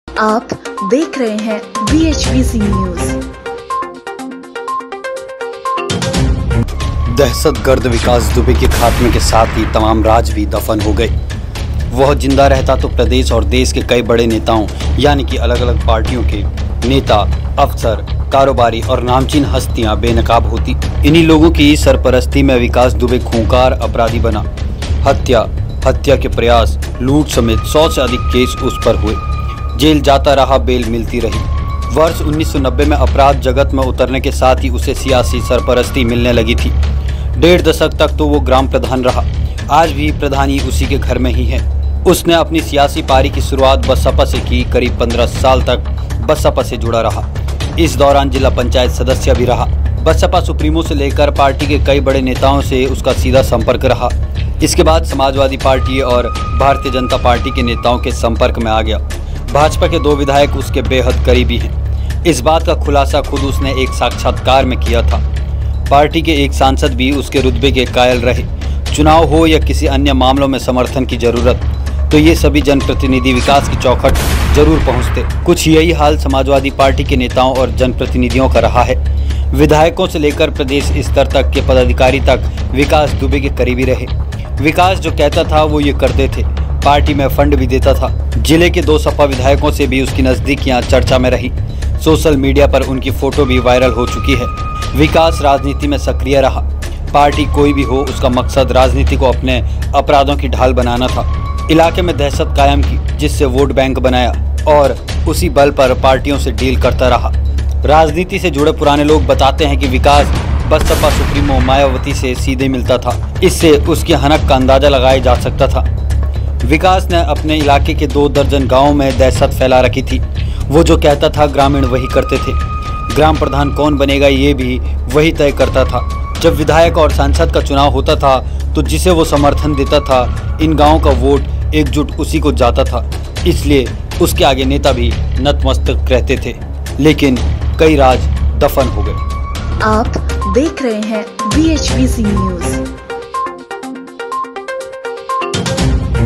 विकास दुबे जैसे अपराधी सत्ता की जरुरत या मज़बूरी ?
दहशतगर्द विकास दुबे के खात्मे के साथ ही तमाम राज भी दफन हो गए। वह जिंदा रहता तो प्रदेश और देश के कई बड़े नेता (अलग-अलग पार्टियों के), अफसर, कारोबारी और नामचीन हस्तियां बेनकाब होतीं। अपराधियों से इनके गठजोड़ की गांठें भी खुलतीं। इन्हीं लोगों की सरपरस्ती में विकास दुबे खूंखार अपराधी बना। हत्या, हत्या के प्रयास, लूट समेत सौ से अधिक केस उस पर हुए। जेल जाता रहा, बेल मिलती रही।